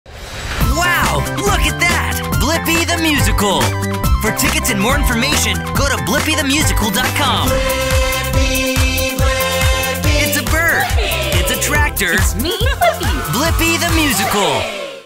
Blippi The Musical | Cheyenne Civic Center
Blippi The Musical brings the energetic and loveable character Blippi off the screen and onto the stage with world-class production, audience engagement, and amazing music.